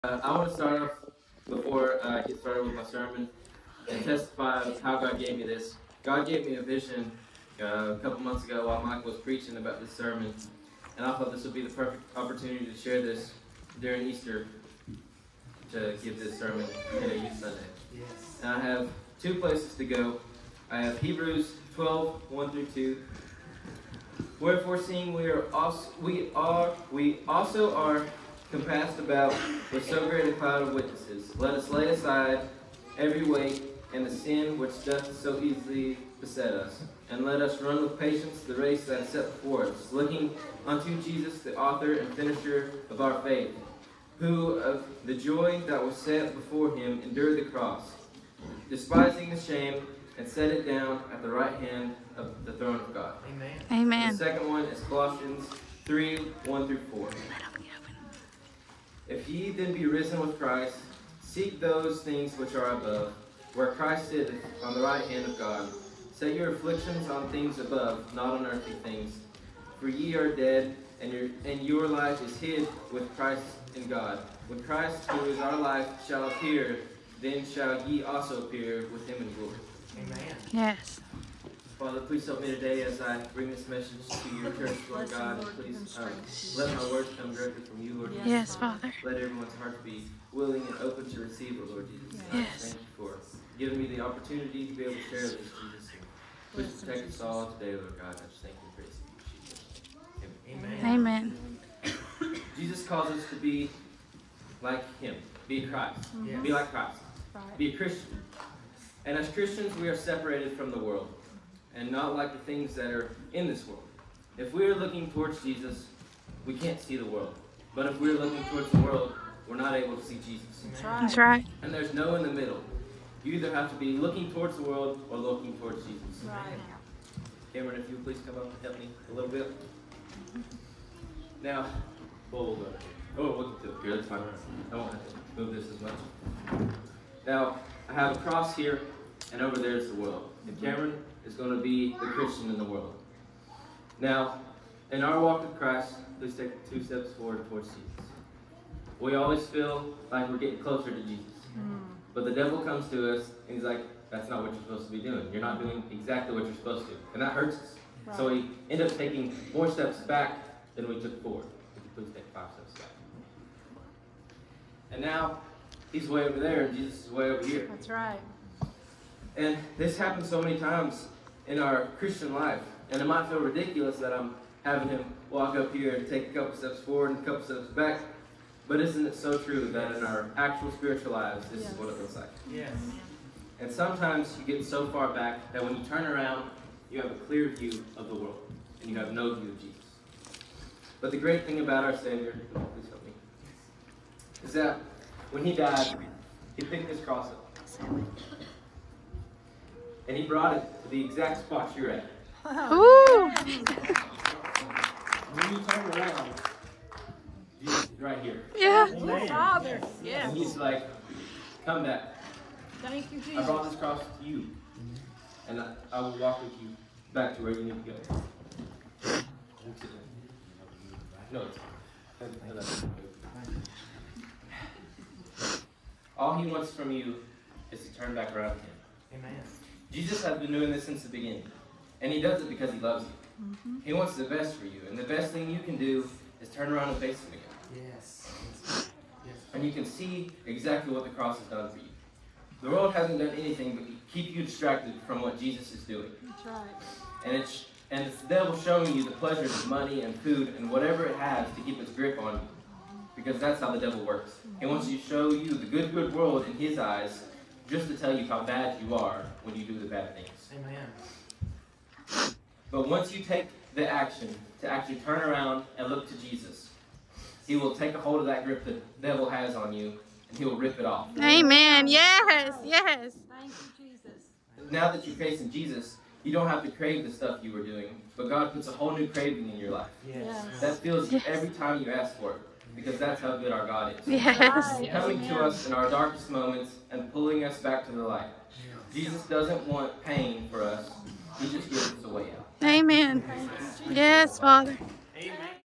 Connor Johnson - The Escape
Do you feel like you are getting further and further from Christ?  Connor talks about how easy it is to turn your life back to Christ.

Support us!